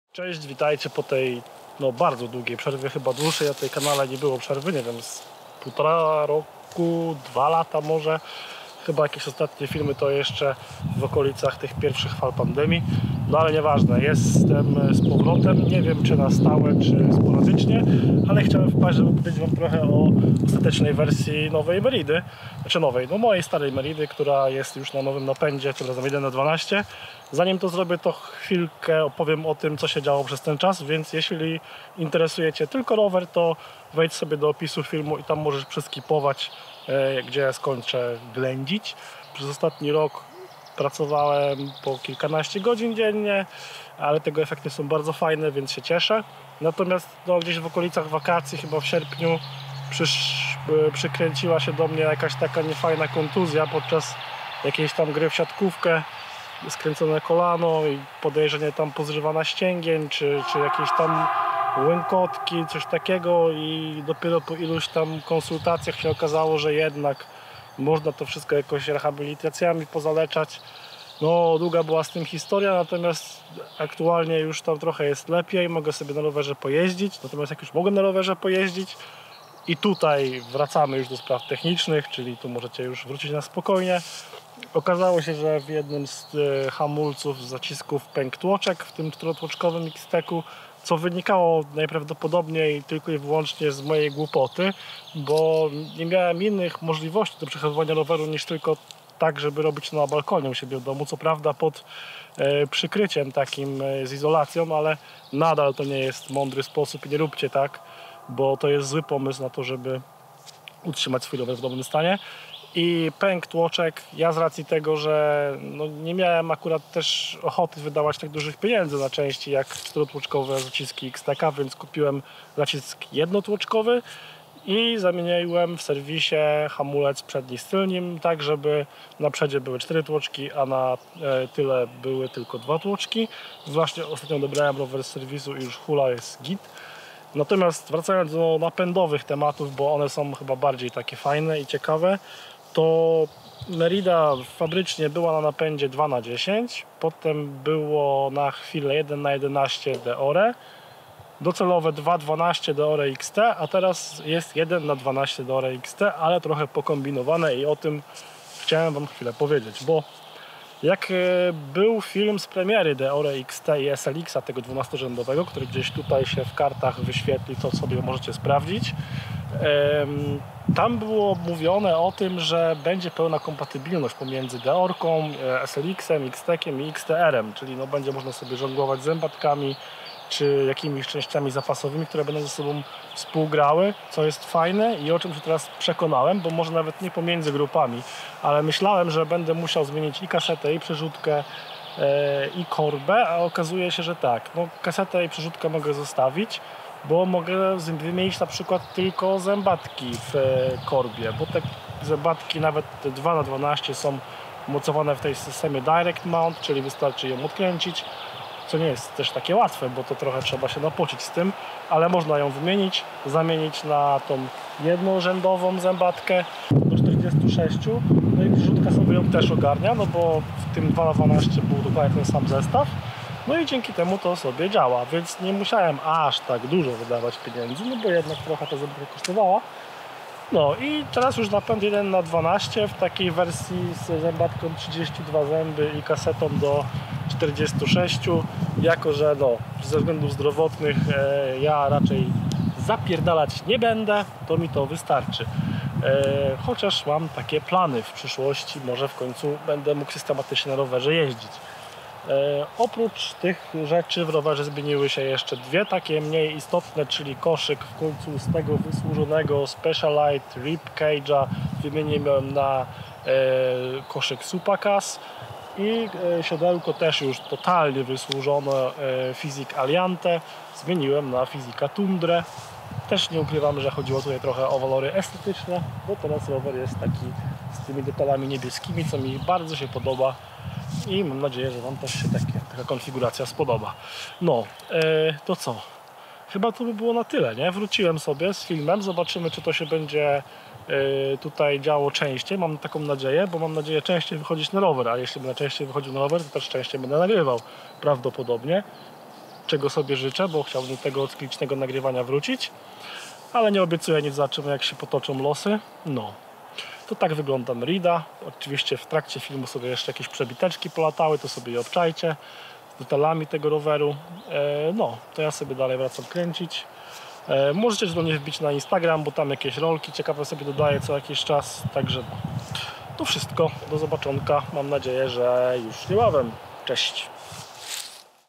Testuję nowy napęd DEORE XT 1x12 w mojej Meridzie
W tym filmie przetestuję nowy napęd DEORE XT 1x12 w mojej ukochanej Meridzie. Czy jest lepszy niż poprzedni napęd 2x12? Zobaczcie sami.

00:00 - 🚀 Wprowadzenie
00:05 - 💤 Przerwa w publikacjach
01:20 - 🔧 Test nowego napędu DEORE XT 1x12

Sprzęt użyty do nakręcenia:
📹 Aparat: Lumix GH6
📷 Obiektyw: Laowa c-dreamer 17mm F1.8
🎤 Mikrofon: BOYA BY PVM1000L